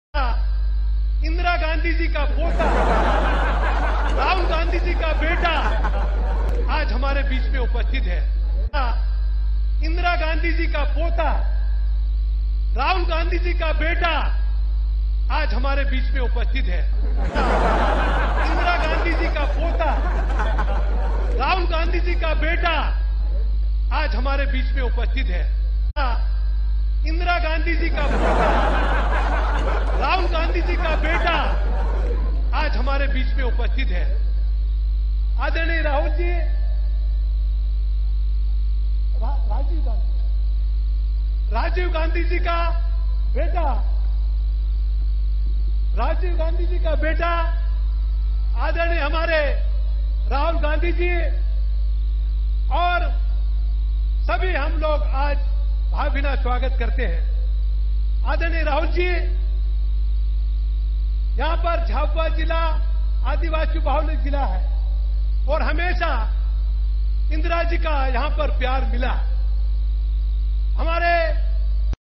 राहुल गांधी का असली बाप कौन, इस कांग्रेस नेता ने बताया!
सोमवार को झाबुआ आये राहुल गांधी की सभा के दौरान कांतिलाल भूरिया की जबान फिसल गई।